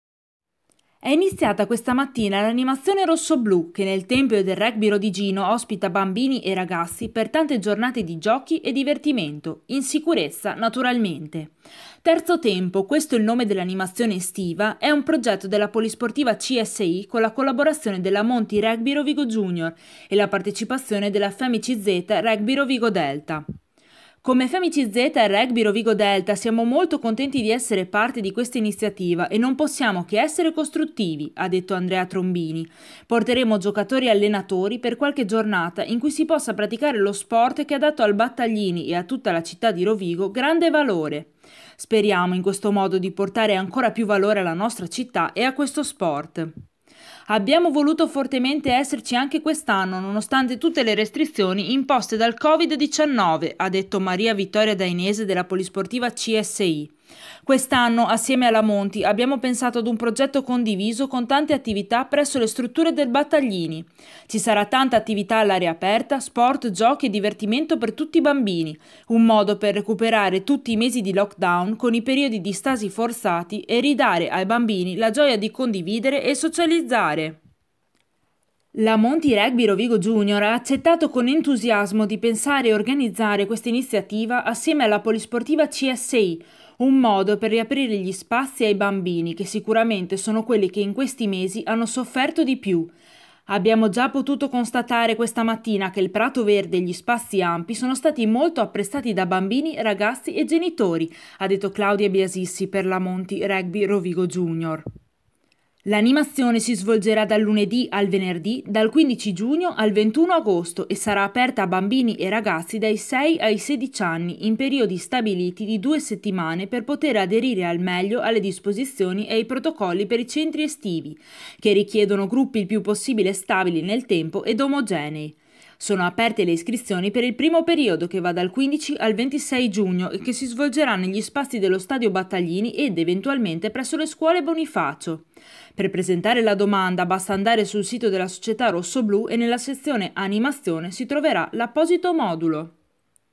Si torna in campo, l'animazione estiva della Monti Rugby Rovigo Junior si fa allo stadio Battaglini
È iniziata oggi l’animazione rossoblù che nel tempio del rugby rodigino ospita bambini e ragazzi, per tante giornate di giochi e divertimento. In sicurezza, naturalmente. “Terzo tempo” - questo il nome dell’animazione estiva - è un progetti della polisportiva CSI con la collaborazione della Monti Rugby Rovigo Junior e la partecipazione della Femi-CZ Rugby Rovigo Delta.

“Come Femi-CZ Rugby Rovigo Delta siamo molto contenti di essere parte di questa iniziativa e non possiamo che essere costruttivi - ha detto Andrea Trombini - Porteremo giocatori e allenatori per qualche giornata in cui si possa praticare lo sport che ha dato al Battaglini e a tutta la città di Rovigo grande valore. Speriamo in questo modo di portare ancora più valore alla nostra città e a questo sport”.

“Abbiamo voluto fortemente esserci anche quest’anno nonostante tutte le restrizioni imposte dal covid-19 - ha detto Maria Vittoria Dainese della polisportiva Csi - Quest’anno, assieme alla Monti, abbiamo pensato a un progetto condiviso con tante attività presso le strutture del Battaglini. Ci sarà tanta attività all’aria aperta, sport, giochi e divertimento per tutti i bambini. Un modo per recuperare tutti i mesi di lockdown con i periodi di stasi forzati e ridare ai bambini la gioia di condividere e socializzare”.

“La Monti Rugby Rovigo Junior ha accettato con entusiasmo di pensare e organizzare questa iniziativa assieme alla polisportiva CSI. Un modo per riaprire gli spazi ai bambini che sicuramente sono quelli che in questi mesi hanno sofferto di più e ne avevano bisogno. Abbiamo già potuto constatare questa mattina che il prato verde e gli spazi ampi sono stati molto apprezzati da bambini, ragazzi e genitori” ha detto Claudia Biasissi per Monti Rugby Rovigo Junior.

L’animazione si svolgerà dal lunedì al venerdì, dal 15 giugno al 21 agosto e sarà aperta a bambini e ragazzi dai 6 ai 16 anni in periodi stabiliti di 2 settimane per poter aderire al meglio alle disposizioni e ai protocolli per i centri estivi che richiedono gruppi il più possibile stabili nel tempo ed omogenei. Sono aperte le iscrizioni per il primo periodo, che va dal 15 al 26 giugno e che si svolgerà negli spazi dello Stadio Battaglini ed eventualmente presso le Scuole Bonifacio, sono ancora aperte. Per presentare la domanda basta andare sul sito della società rossoblù e nella sezione ‘animazione’ si troverà l’apposito modulo.